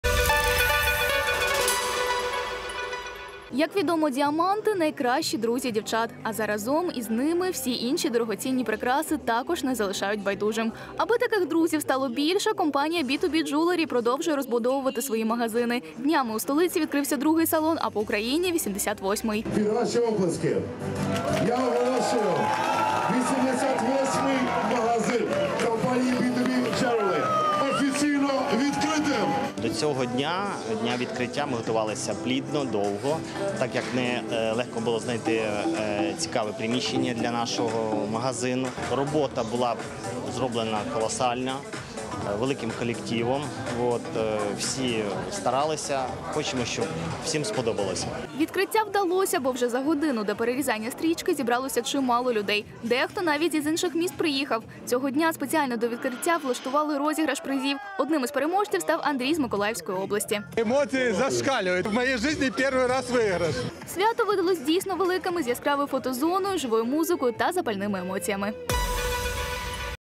Открытие магазина B2B Jewelry: подарки, фотозоны и живая музыка
Как известно бриллианты - лучшие друзья девушек. А заодно с ними - остальные драгоценные украшения также не оставят равнодушными. Чтобы таких друзей становилось больше, компания B2B Jewelry продолжает развивать свои магазины. На днях в столице открылся второй салон, а по Украине - 88-й.